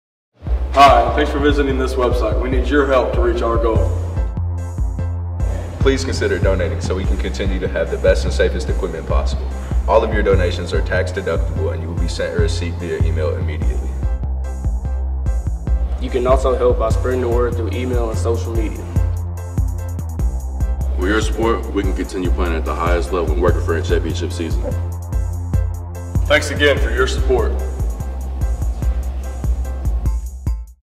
Blackman Football Fundraiser commercial 2020
Help our program be even more successful by donating today to our Liftathon Fundraiser!